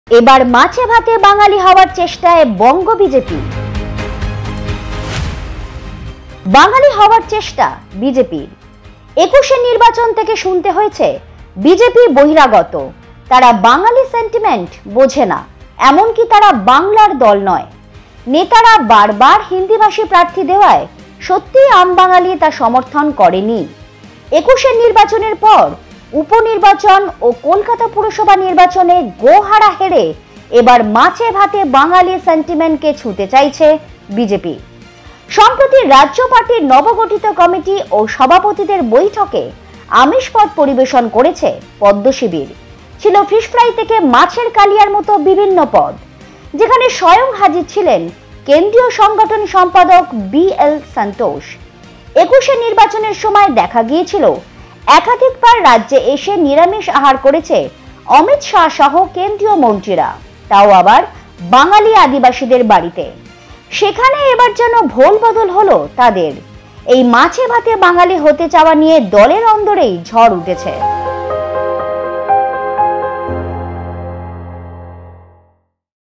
এবার মাছে–ভাতে বাঙালি হওয়ার চেষ্টায় বঙ্গ বিজেপি
Bengal BJP now trying to adopt a rice-and-fish Bengali way to halt its rapidly deteriorating fortunes in the state